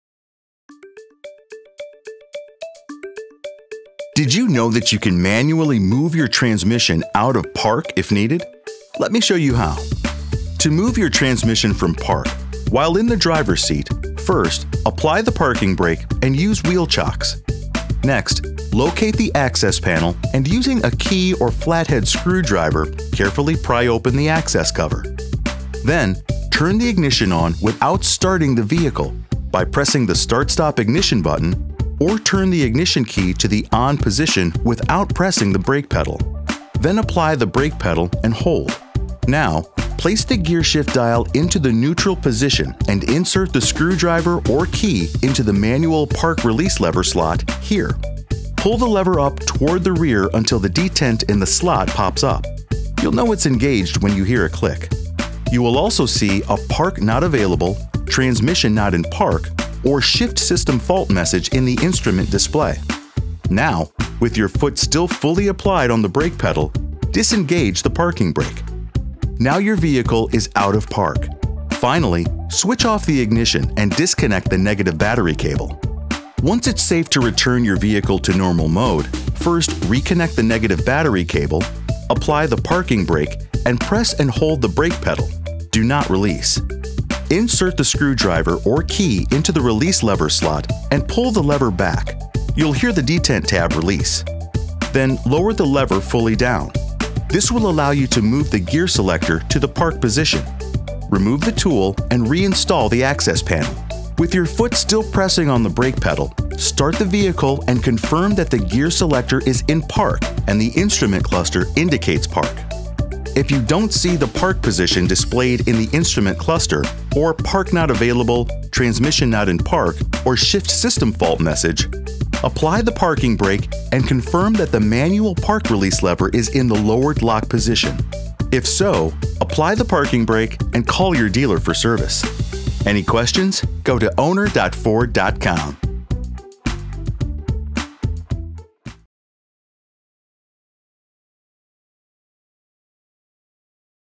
Manual Park Override | Ford How-To
This easy-to-follow video will show you how to take your vehicle out of Park if you encounter an electrical failure or dead battery and need to have the vehicle towed.*

*Computer-generated images shown.